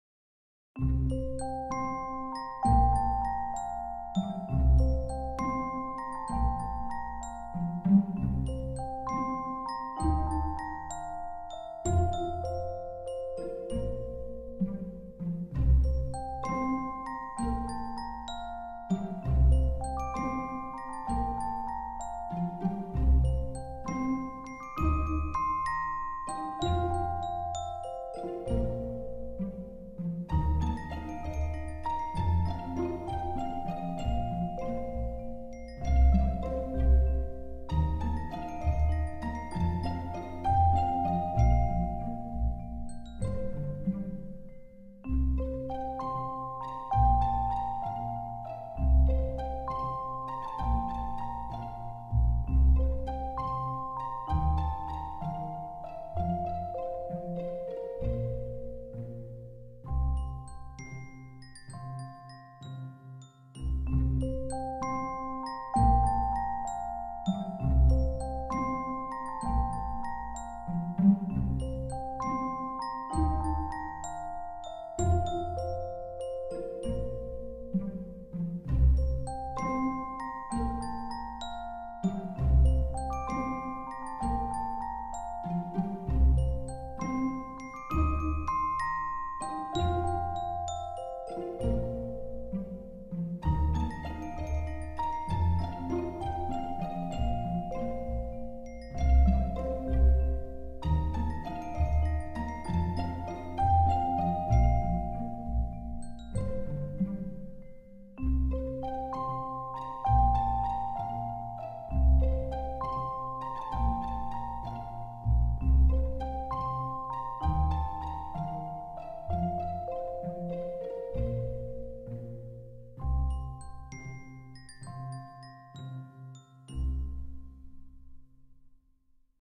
Where do Fairies Live? Oh Look!
Where do fairies live?  Why, they live in trees and mushrooms deep in the forest...AI helped me to create these beautiful fairy villas.   I hope you enjoy their magical world.